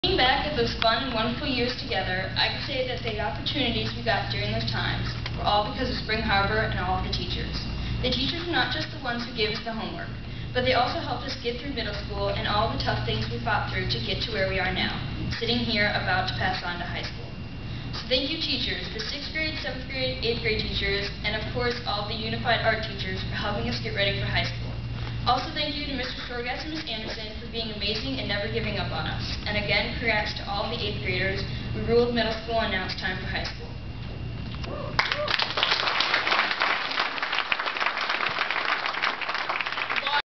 Monika's graduation speech...
Monika was one of four students to give the end of Middle School graduation speech.